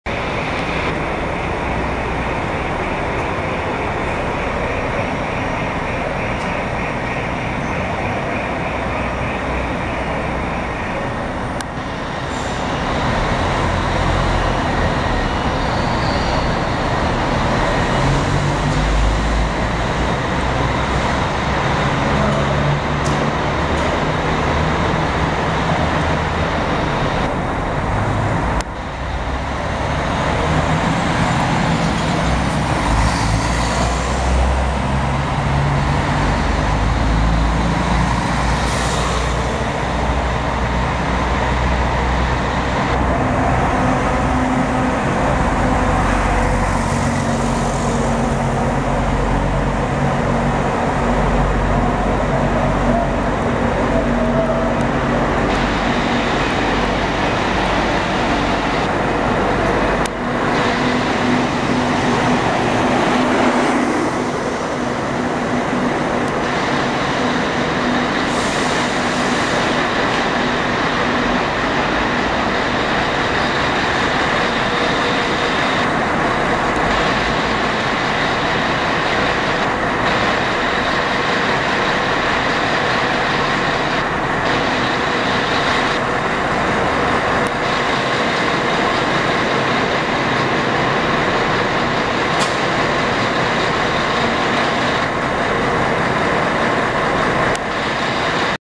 CMA FLECHA AZUL 7455 *VIAÇÃO COMETA 65 ANOS*
12 DE SETEMBRO DE 2013
VIGÉSIMA VIAGEM, LORENA X SÃO PAULO
CHEGADA NO TERMINAL TIETÊ SP.
VIAGEM COMEMORATIVA DO FLECHA AZUL 7455 "65 ANOS VIAÇÃO COMETA S/A"